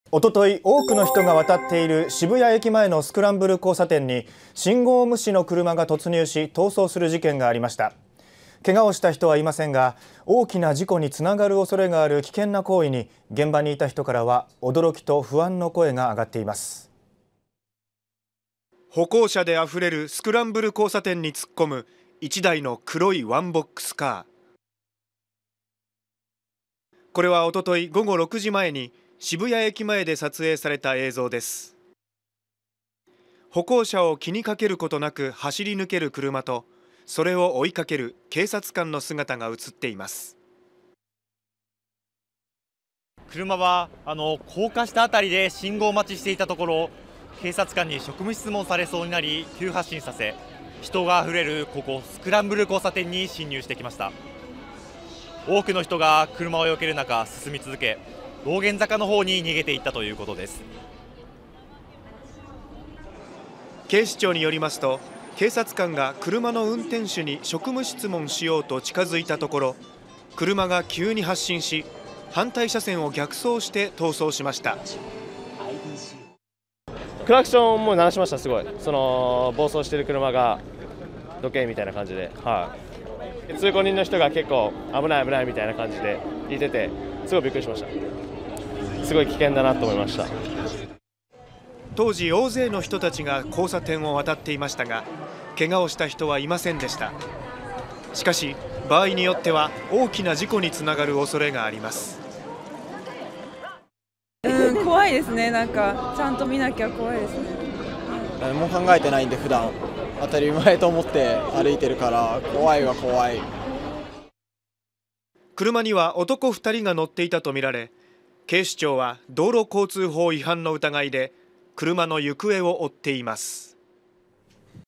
東京・渋谷のスクランブル交差点に車が突入
4月7日の午後6時前、多くの人が渡っている東京の渋谷駅前にあるスクランブル交差点に、信号無視の車が突入し、逃走する事件がありました。けがをした人はいませんでしたが、大きな事故につながる恐れがある危険な行為に、現場にいた人からは驚きと不安の声が上がっています。

　渋谷駅前で撮影された映像には、歩行者を気にかけることなく、人であふれるスクランブル交差点に突っ込み走り抜ける1台の黒いワンボックスカーと、それを追い掛ける警察官の姿が写っています。車は多くの人が避ける中を進み続け、道玄坂の方向に逃走しました。

　警視庁によりますと、渋谷駅前の高架下で信号待ちをしていた車の運転手に警察官が職務質問しようと近づいたところ、車は急に発進し、反対車線を逆走して逃走しました。目撃者は「暴走している車が『どけ』みたいな感じでクラクションをすごく鳴らしていた。通行人が『危ない危ない』みたいに言っていて、びっくりした。すごく危険だと感じた」と話しました。当時、大勢の人たちが交差点を渡っていましたが、けがをした人はいませんでした。

　しかし、場合によっては大きな事故につながる恐れがあります。街の人は「怖いですね。ちゃんと見ていないと怖い」「普段、何も考えずに当たり前だと思って歩いているから怖い」などと話していました。

　車には男2人が乗っていたとみられ、警視庁は道路交通法違反の疑いで車の行方を追っています。